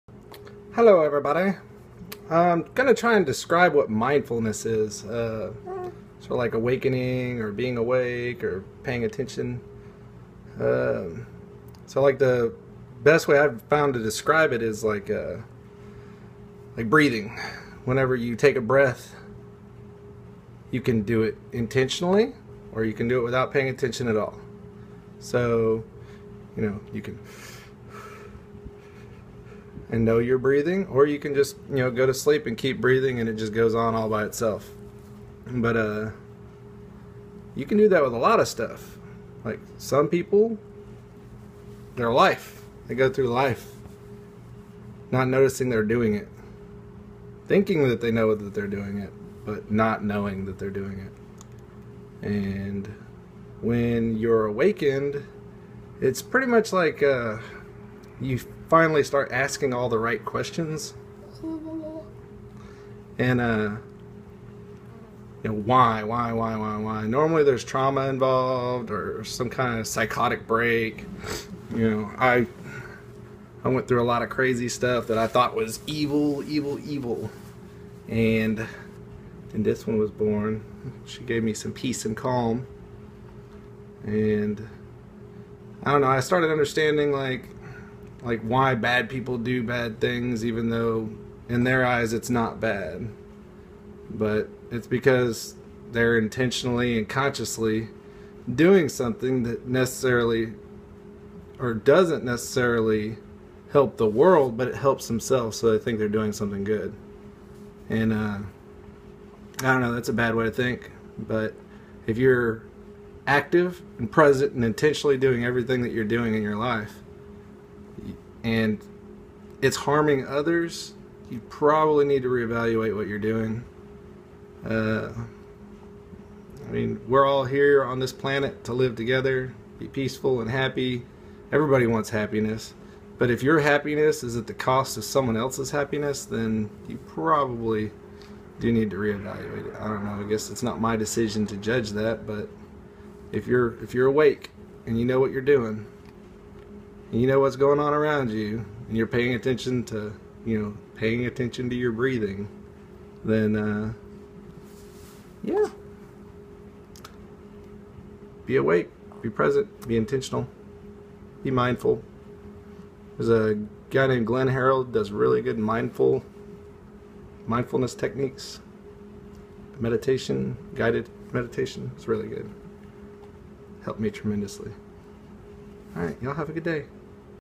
mindfulness
trying to discribe what mindfullness is not the great at it but ill keep trying